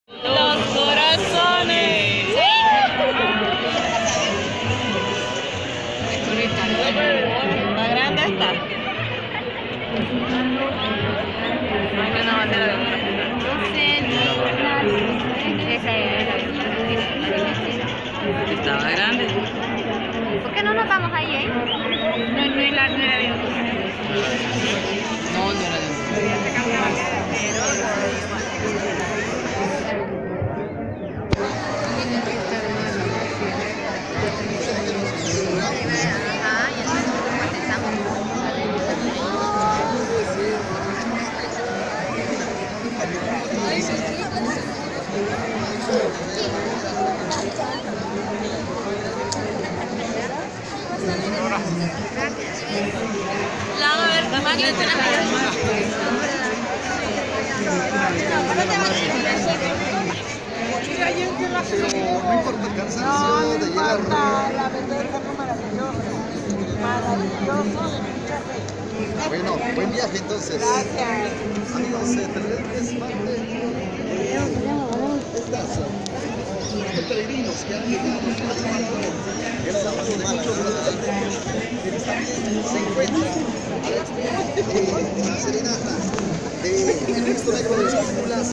Noti Siete 7 Y Marvin García En Alborada del Cristo Negro Esquipulas 2020
Procesión de peregrinos En Ingreso Ala basílica de Cristo NEGRO de Esquipulas Cientos de peregrinos de México ingresan al templo sagrado Antes de la media noche con cantos Y algarabias en este año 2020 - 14 de enero